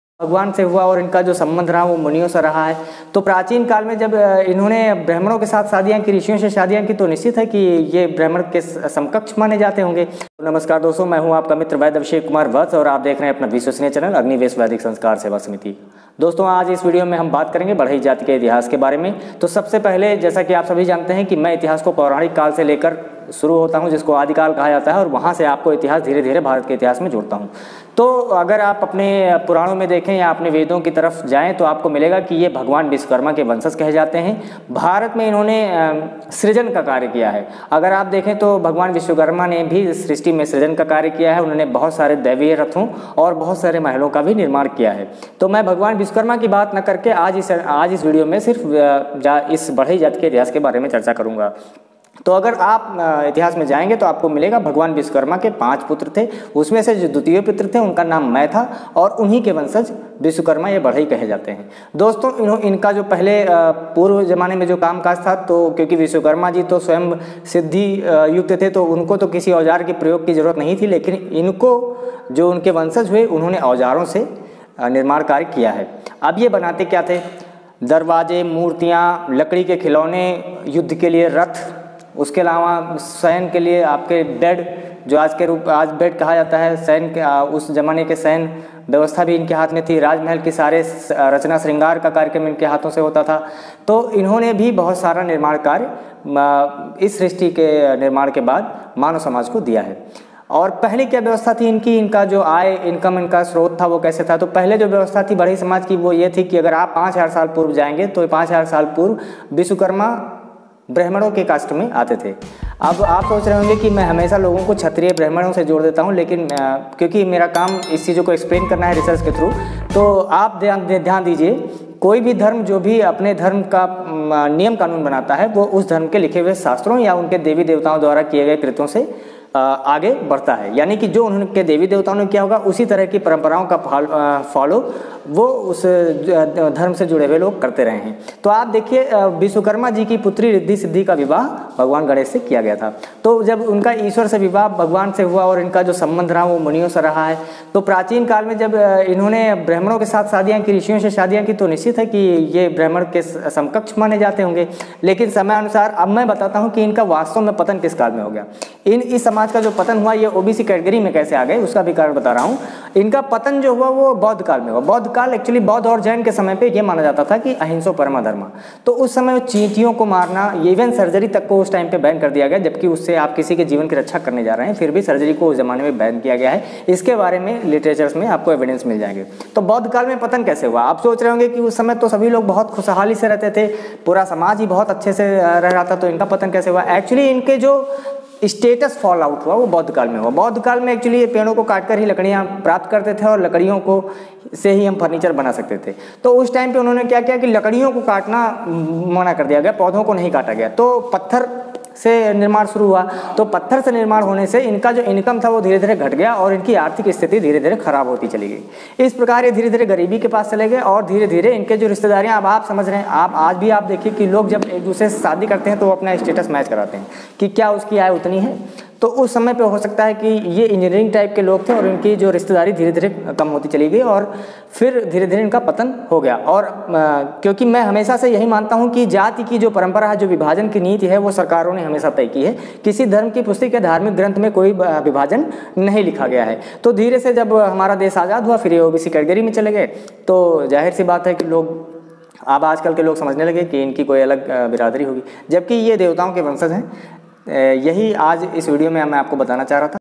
BADHAI JAATI KA ITIHAAS,VISHWAKARMA,
Is video me ham badhai jati ke itihaas par charcha kar rahe hain,yah jaati bahut mehnati hoti thi inki imaandaari ki charcha bhi itihaas ke panno me darj hai..Mehnat kash jaati kalantar me piche rah gayi...Hame inko saath lena hai..

Facebook-vaidya abhishek kumar vatsya